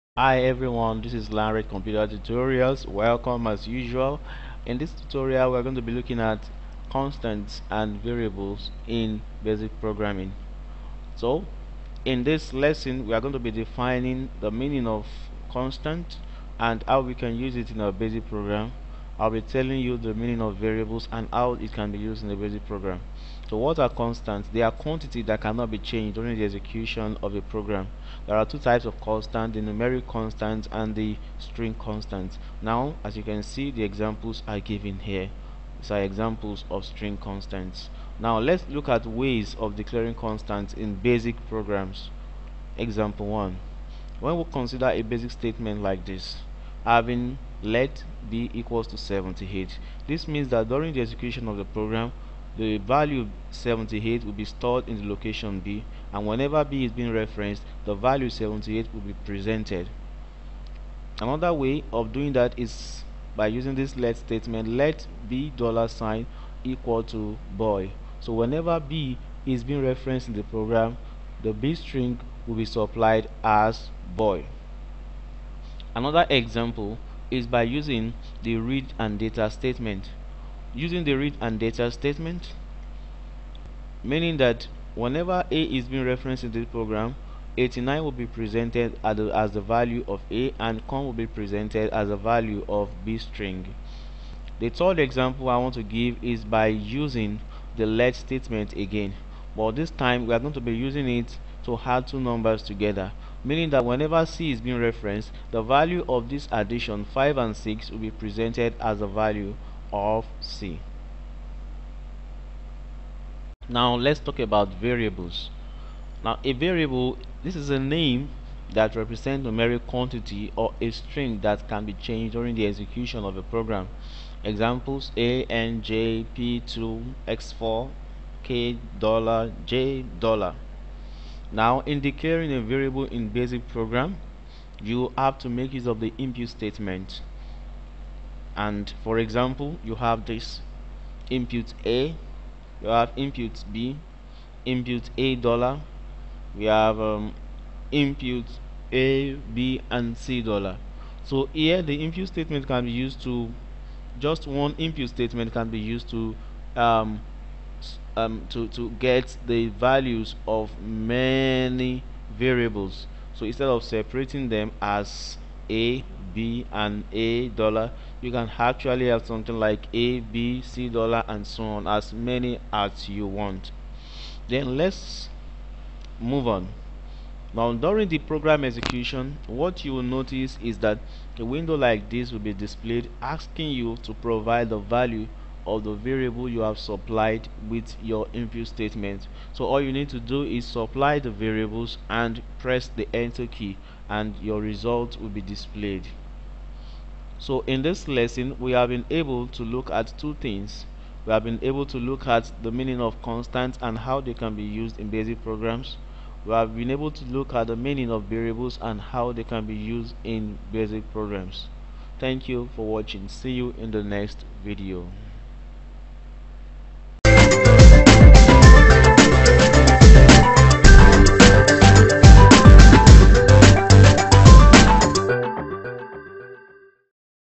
BASIC Programming (Constants & Variables)
This video will show you the use of constants and variable in BASIC Programs.